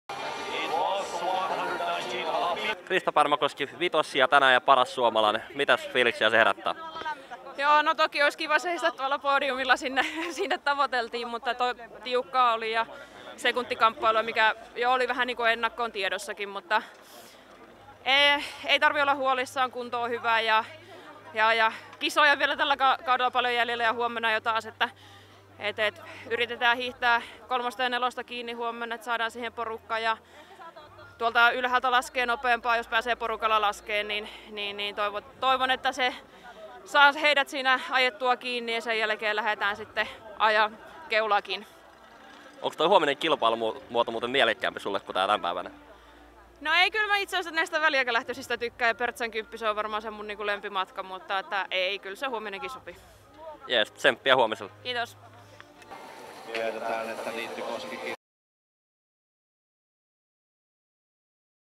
Krista Pärmäkoski hyvistä asemista kohti huomista takaa-ajoa
Krista Pärmäkosken mietteitä lauantain 10km perinteisen jälkeen.